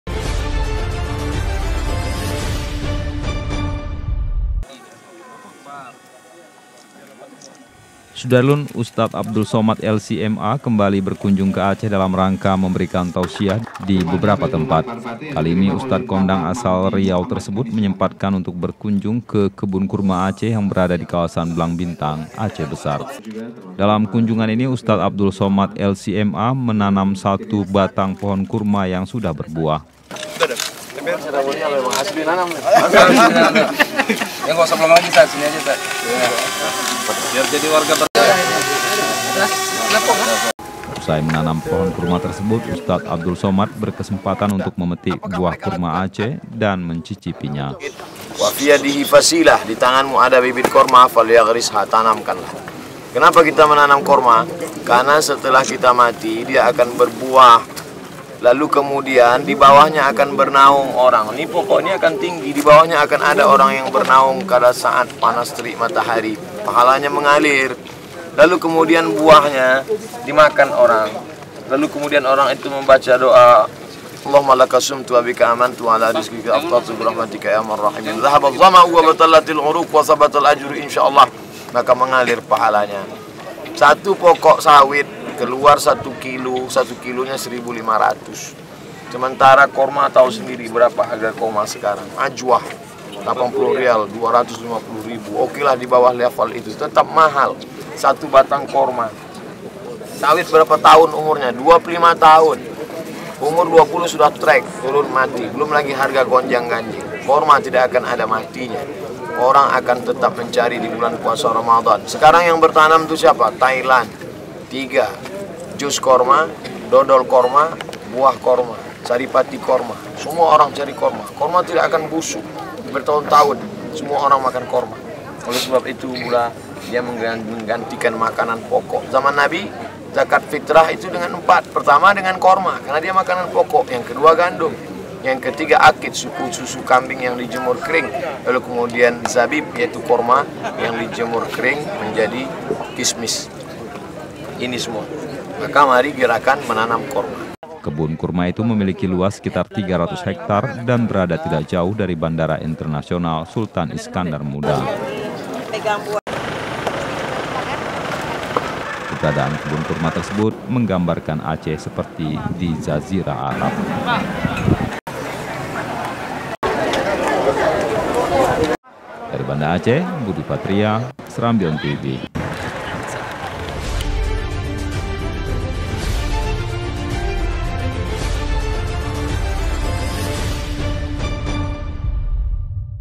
Ini Manfaat Kurma Menurut Penjelasan Ustaz Abdul Somad
SERAMBITV.COM, BANDA ACEH - Ustaz Abdul Somad Lc MA, kembali berkunjung ke Aceh dalam rangka memberikan tausiah di beberapa tempat. Kali ini, ustad kondang asal Riau tersebut menyempatkan untuk berkunjung ke kebun kurma Aceh yang berada di kawasan Blangbintang Aceh Besar, Senin (3/7/2018).

Dalam kunjungan ini Ustaz Abdul Somad Lc MA, menanam satu batang pohon kurma yang sudah berbuah. Usai menanam pohon kurma tersebut, Ustaz Abdul Somad, berkesempatan untuk memetik buah kurma Aceh dan mencicipinya.

Kebun kurma itu memiliki luas sekitar 300 hektar dan berada tidak jauh dari Bandara Internasional Sultan Iskandar Muda. Keberadaan kebun kurma tersebut menggambarkan Aceh seperti di Zazirah Arab.